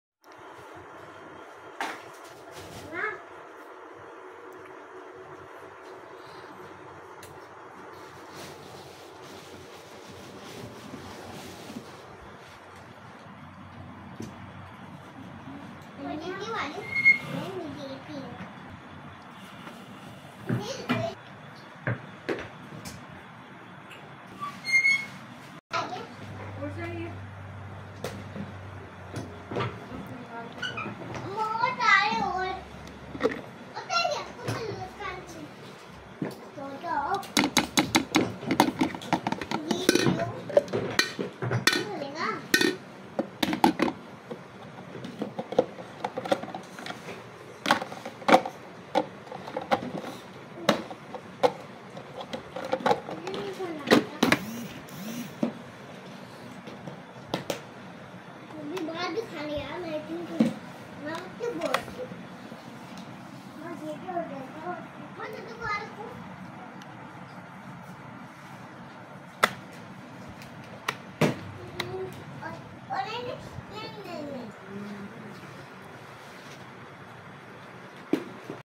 Brand: PrincessPrincess Royal Twin Elite
Brand: Princess

Princess Royal Twin Elite
Type 2182

Coffeemaker : 220-240V /  1000W 10-12 cups

Water Kettle : 220-240V/ 1400W 1.5 L.

Princess 252182 2-in-1 Coffee Maker and Electric Kettle

Price Rs: 14500 with Dc

Whtsapp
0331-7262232